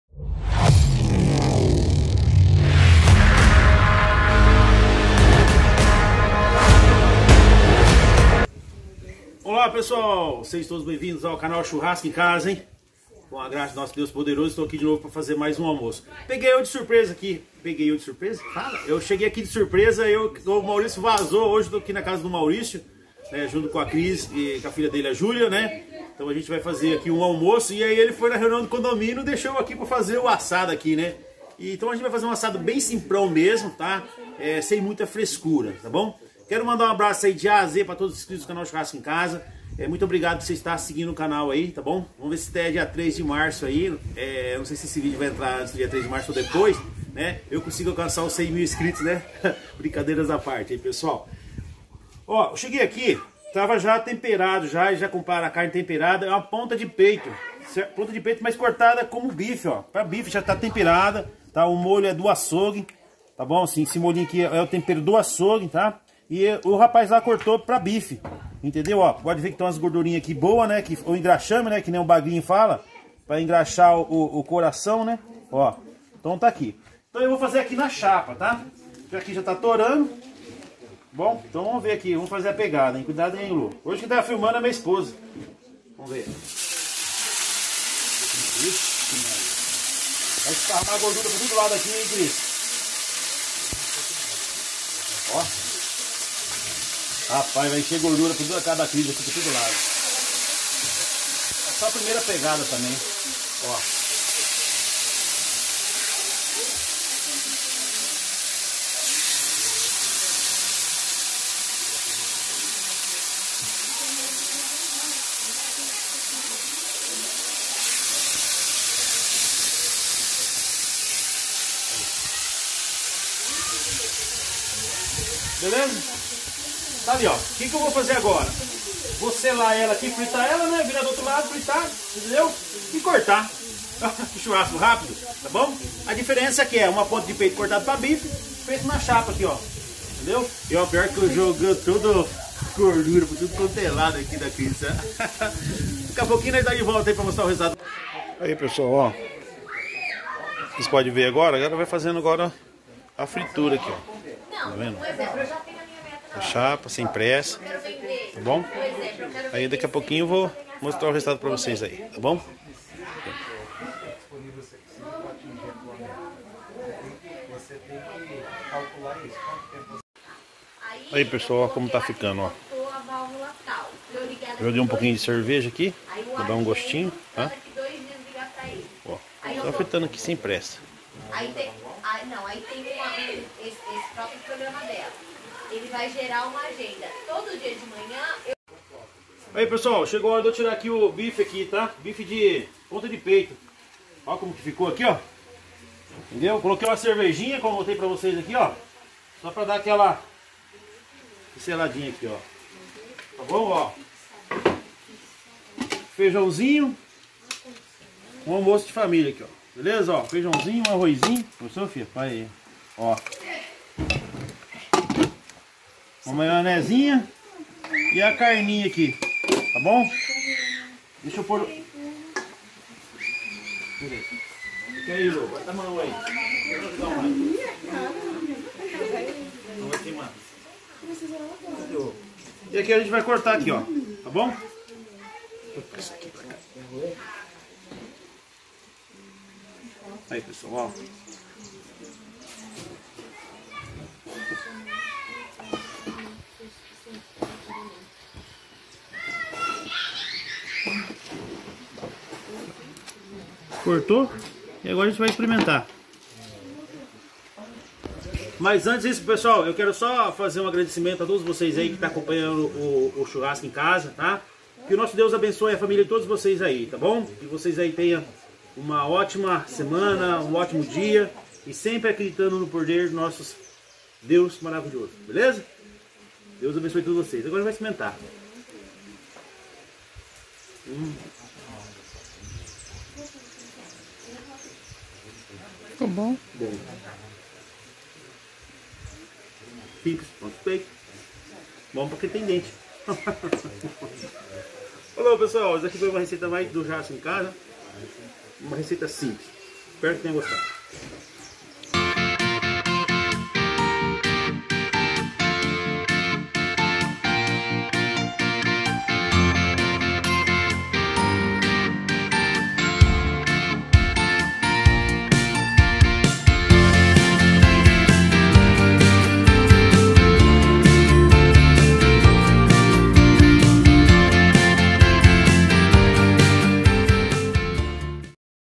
Bife de ponta de peito feito no disco de ferro /Com Renato Monteiro
Olá pessoal quero deixar aqui meu abraços a todos os inscritos que nosso senhor de muitas bênção a todos.

Hoje receita foi bije de ponta de peito feito no disco de ferro fui pego de surpresa não seria eu que ia fazer a receita mas acabou eu indo para churrasqueira.
Receita Bife da Ponta de Peito o tempero já foi feito pelo açougue só coloquei cerveja para dar um gosto especial.
Galera nesse estilo carne ficou dura mas bem saborosa isso que importa kkkk e era só isso que tinha para comer rsss não sobrou foi nada.
Galera me siga no instagram @churrascoemcasarenatomonteiro.